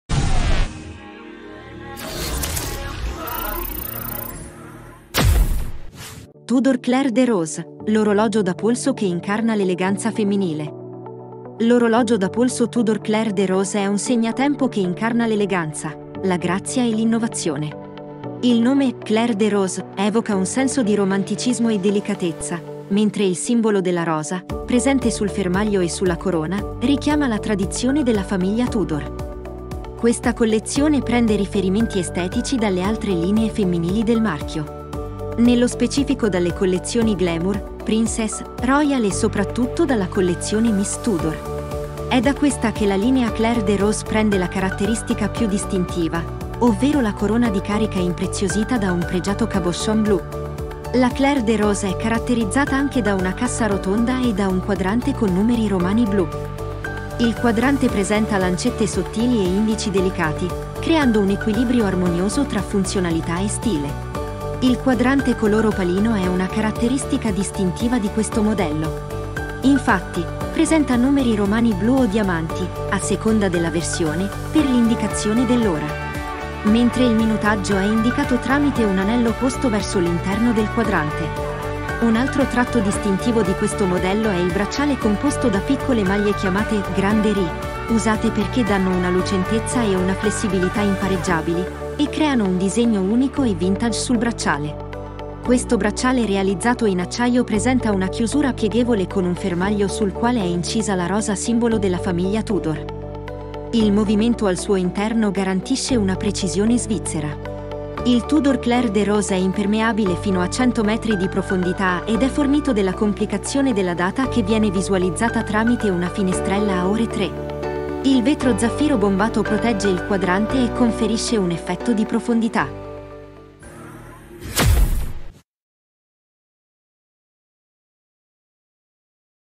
TUDOR CLAIR DE ROSE: l’orologio da polso che incarna l’eleganza femminile
Scopri il modello Tudor Clair de Rose, l’orologio da polso pensato per il genere femminile che combina eleganza, grazia e innovazione. Il Tudor Clair de Rose è un orologio che racconta una storia, quella della famiglia Tudor e della sua tradizione orologiera. Il Tudor Clair de Rose è un orologio che celebra la rosa, il simbolo della casa Tudor e della sua origine storica.
È un orologio che ti regala emozioni, sensazioni e ricordi. Il Tudor Clair de Rose è più di un orologio, è un gioiello, è un’opera d’arte, è un capolavoro.Esplora la sua storia, il suo design e le sue caratteristiche tecniche.